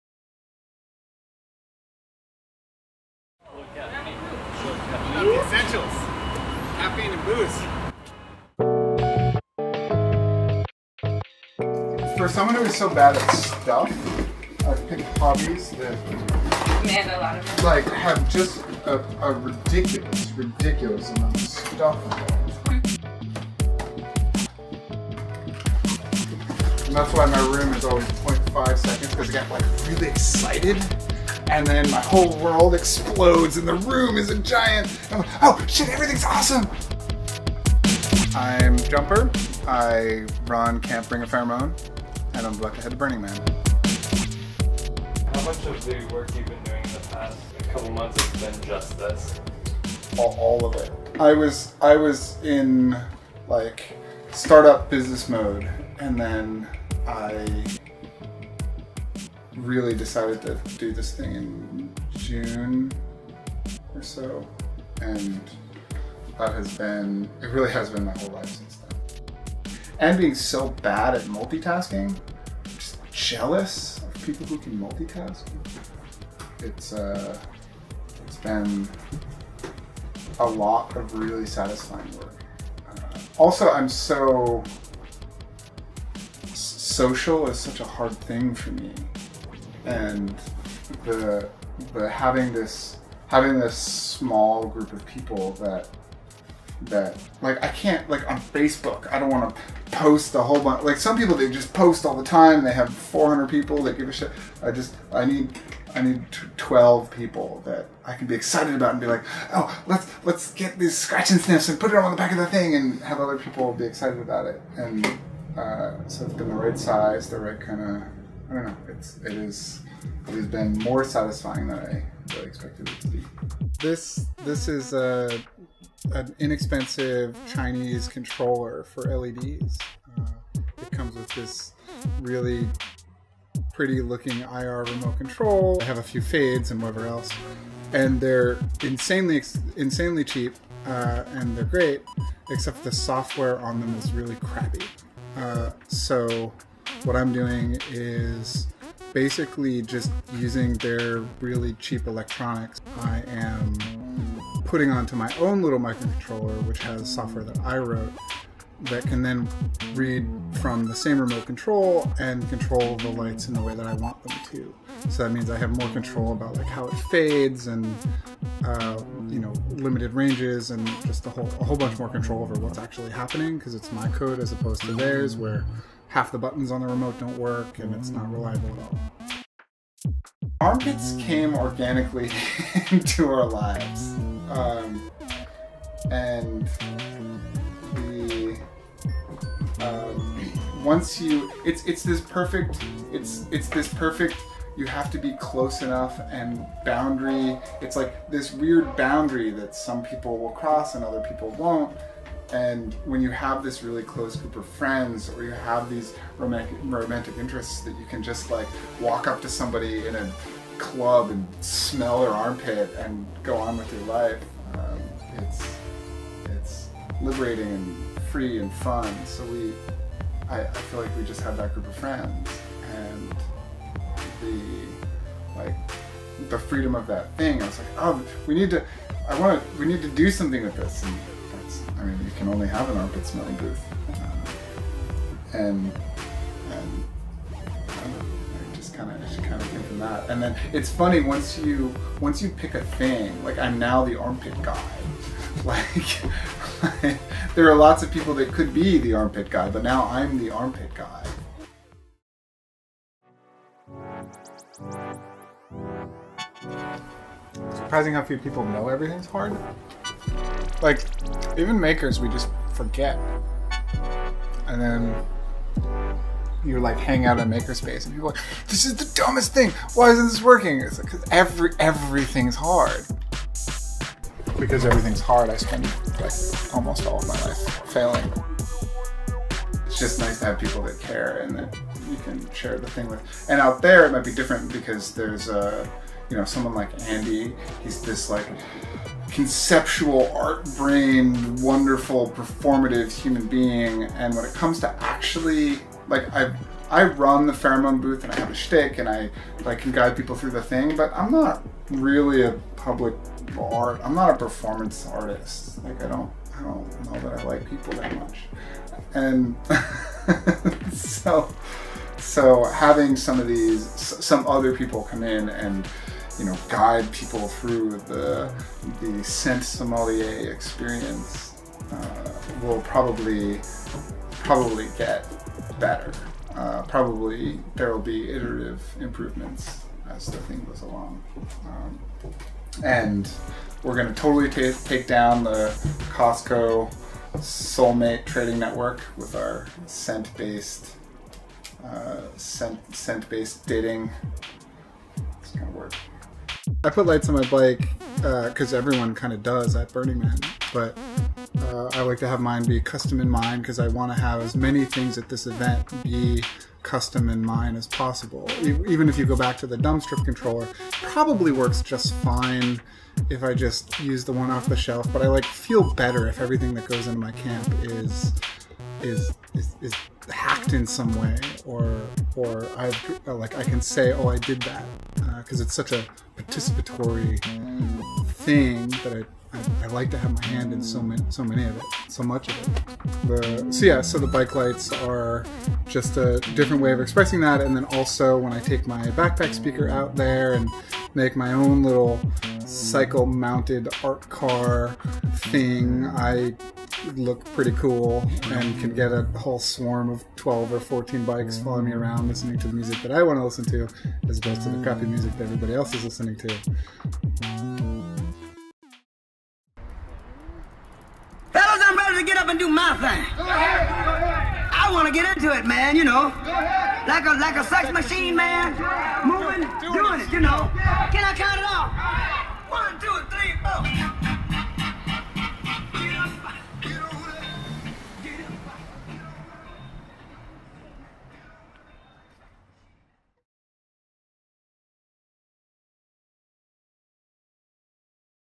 A Place And All Its Creatures: This is Jumper
A short piece about Jumper, our friend who heads Camp Bring A Pheromone at Burning Man. This is some of the work he's been doing as preparation for Burning Man 2015